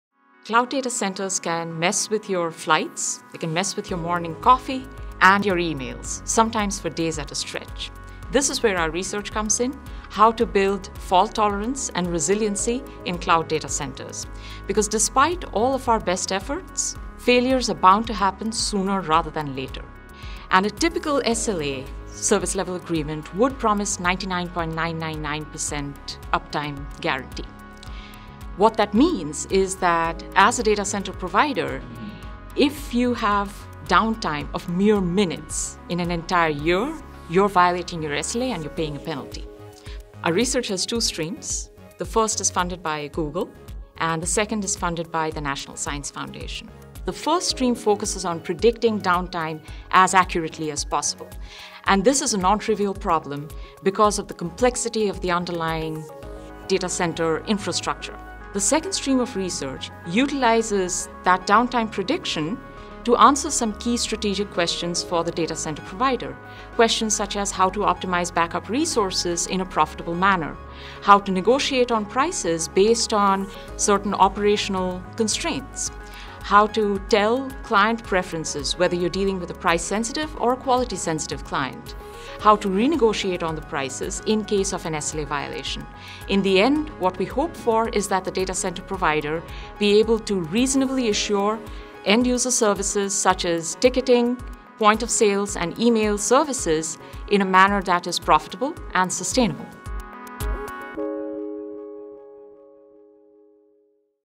Making cloud computing more reliable - Two Minute Tuesdays
Sanjukta Smith, associate professor of management science and systems in the University at Buffalo School of Management, on how to make cloud computing more reliable (co-authored with Anna Ye Du, Chunming Qiao, R. Ramesh and Zhouhan Yang). Learn more about the UB School of Management